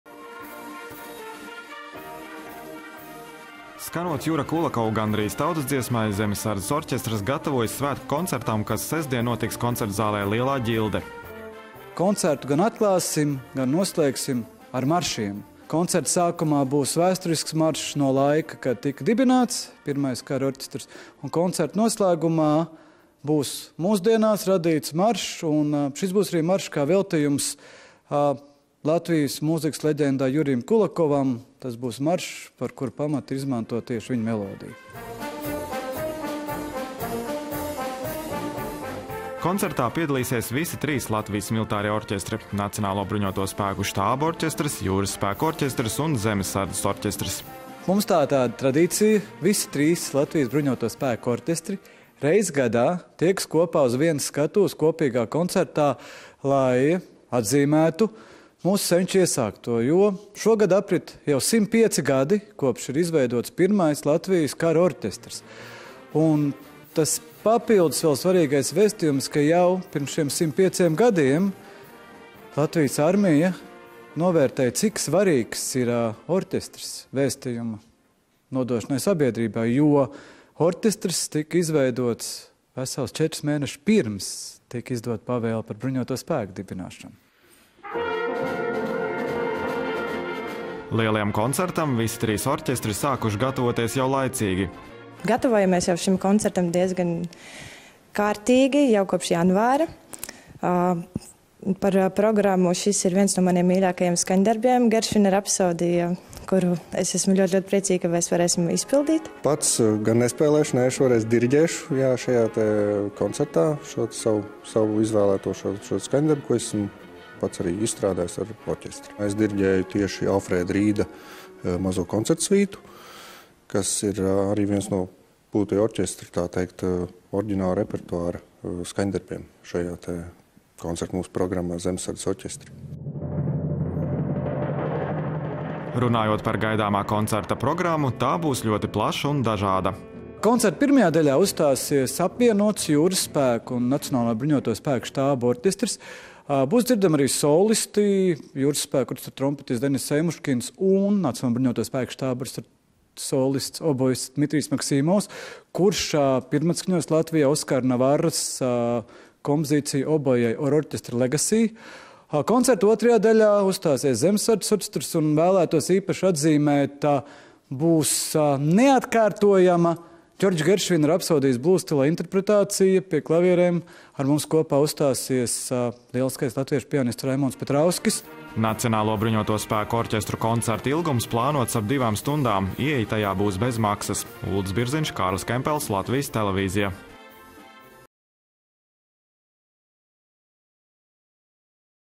Svinot 105 jubileju, aicina uz bezmaksas Latvijas militāro orķestru koncertu Lielajā ģildē
Sestdienas vakarā, 9. martā, visi ir laipni aicināti uz Latvijas militāro orķestru koncertu Lielajā ģildē. Šogad aprit 105 gadi, kopš ir izveidots pirmais Latvijas kara orķestris. Par godu tam bezmaksas koncertu vairāk nekā divu stundu garumā sniegs trīs Latvijas militārie orķestri.